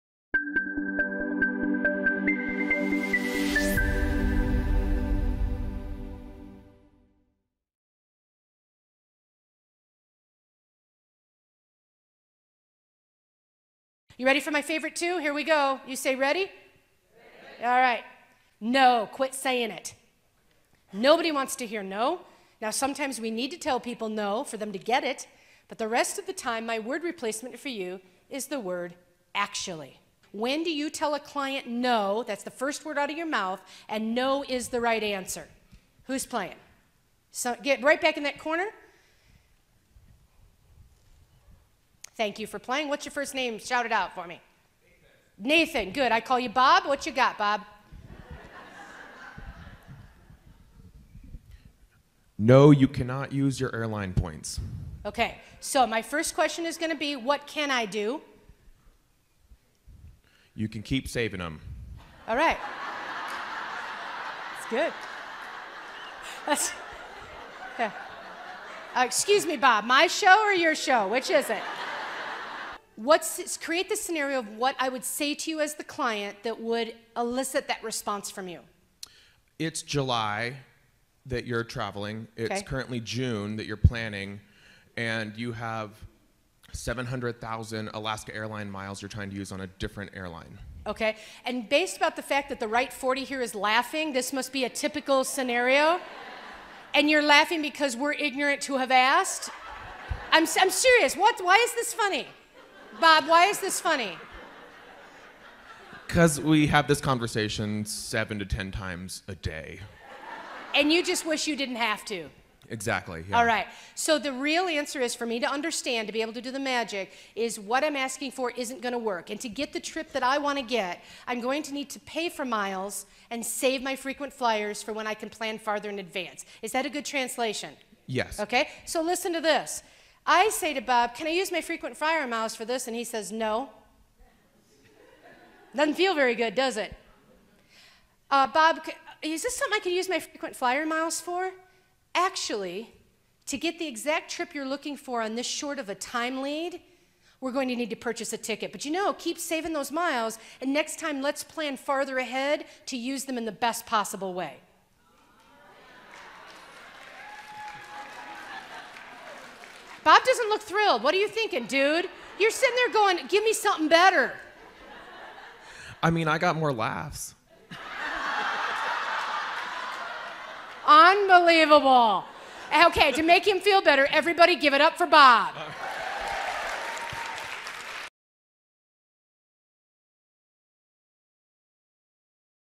Customized to Speak to You | Laurie Guest
For over 26 years, Laurie Guest has built a reputation for being a speaker who does not disappoint. During a successful career in the healthcare industry, she was known as a “go-to-resource” for customer service excellence. She channeled that expertise into Guest Enterprises, Inc., her own speaking and training company and for more than two decades has shared her practical point of view on customer service and staff development to audiences across the country, blending real-life examples and proven action steps for improvement.